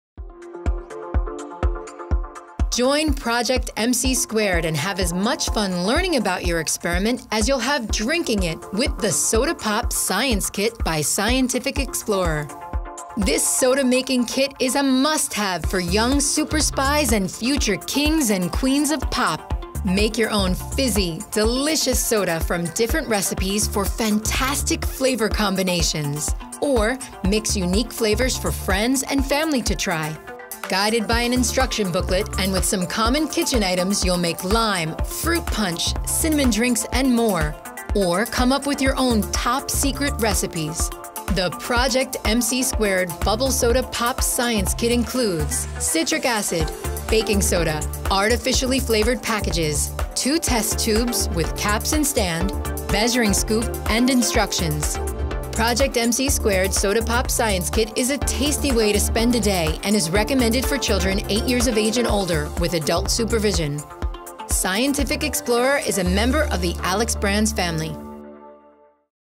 Project MC2 Soda Pop Science Kit
► Project MC2 Soda Pop Science Kit lets future kings and queens of pop have as much fun drinking their experiment as they had learning the science behind it.